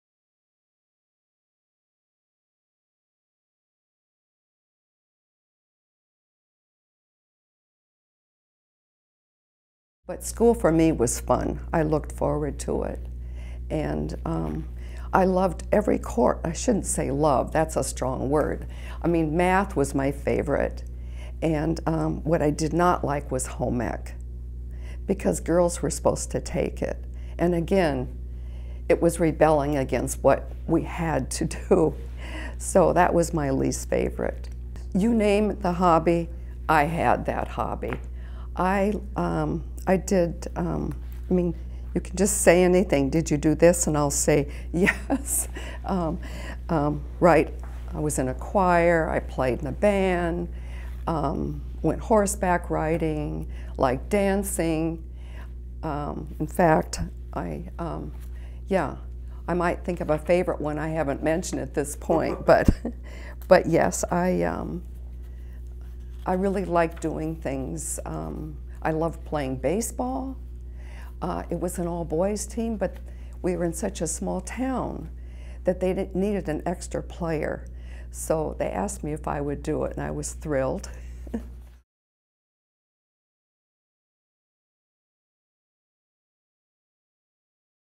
Math and Baseball
Margaret Hamilton describes her favorite classes and hobbies as a youth.